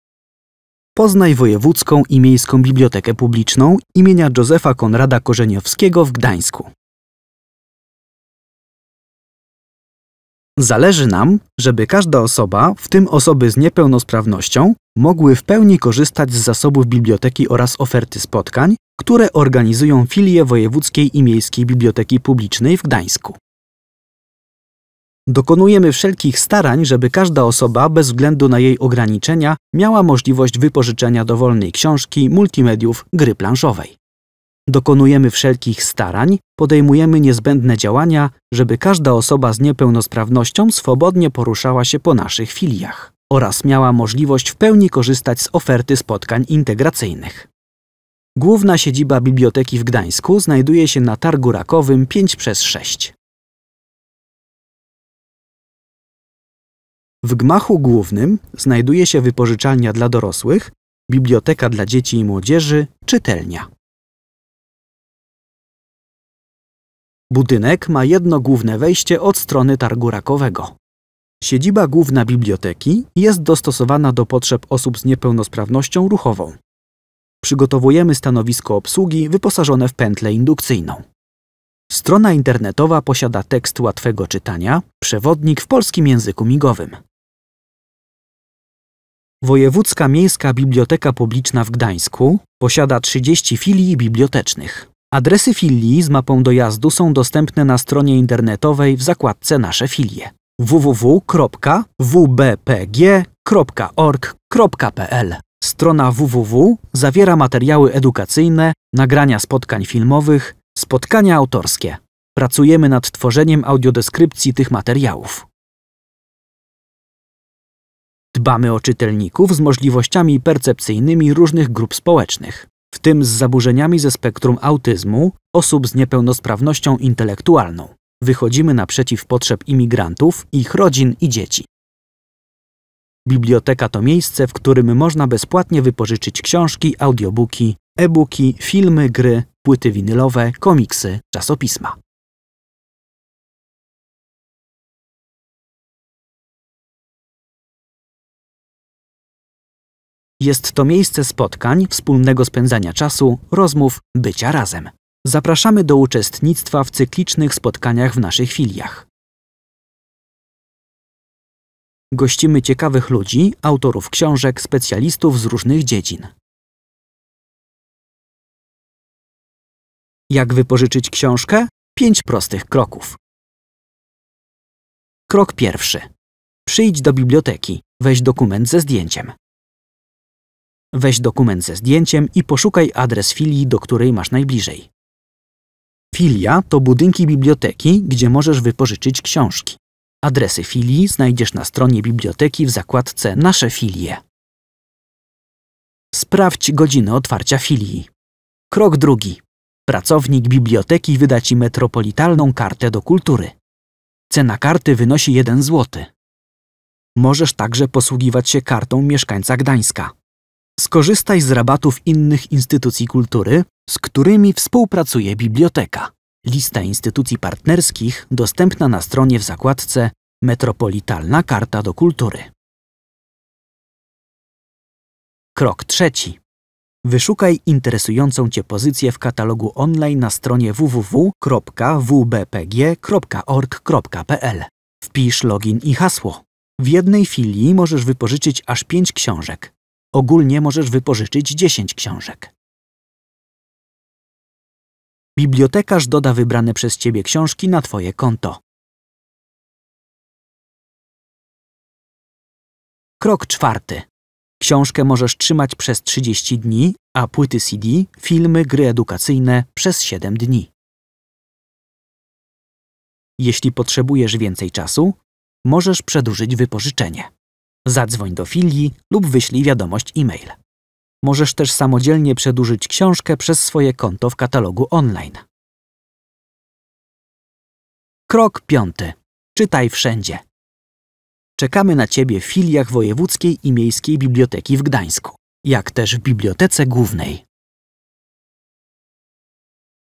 Przedprzewodnik po WiMBP Gdańsk - polski język migowy (PJM) oraz lektor - Biblioteka Gdańsk
Przygotowaliśmy film, tzw. przedprzewodnik, by przekazać niezbędne informacje o dostępności i barierach w budynkach (filiach) Wojewódzkiej i Miejskiej Biblioteki Publicznej im. Josepha Conrada Korzeniowskiego w Gdańsku, planowaniu wizyt oraz najważniejszych zasadach korzystania z zasobów książek, multimediów, gier planszowych i innych zbiorów WiMBP Gdańsk.
Film również z lektorem.